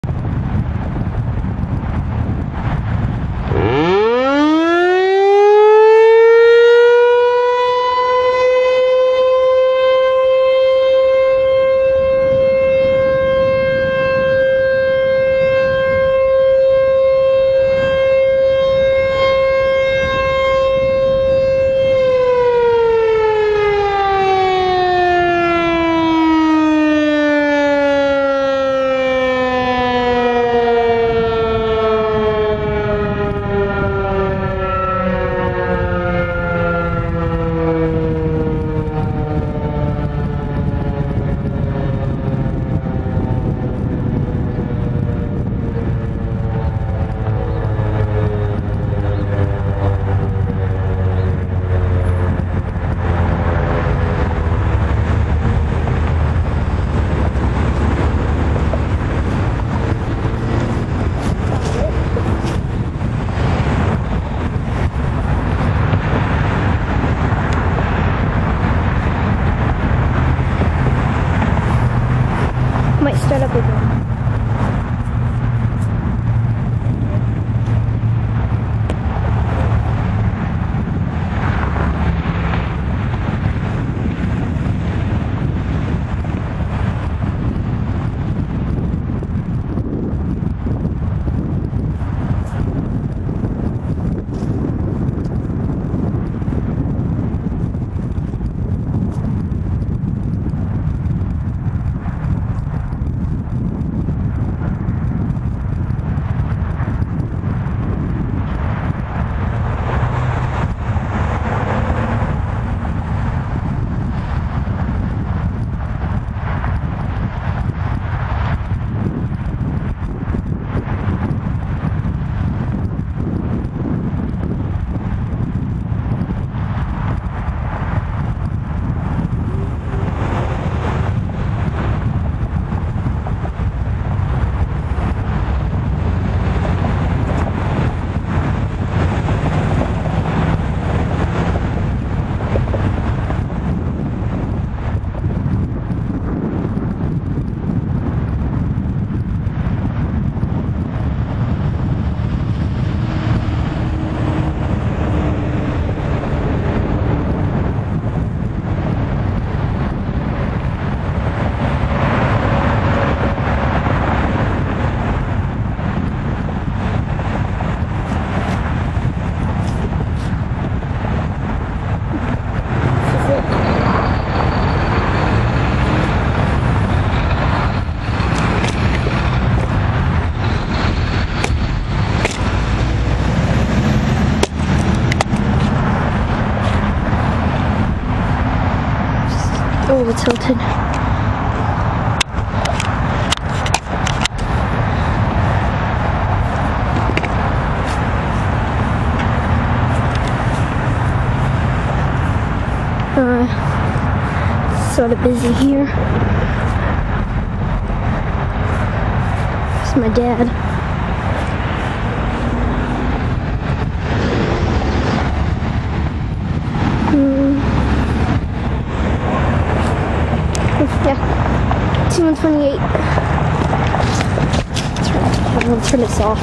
Canton, MI American signal T-128
Canton, Mi ASC T-128 tested at 1:00 PM first Saturday of the month

©⨉ Public Domain
Blixt1000 - Redi Wolf; No Rights Reserved!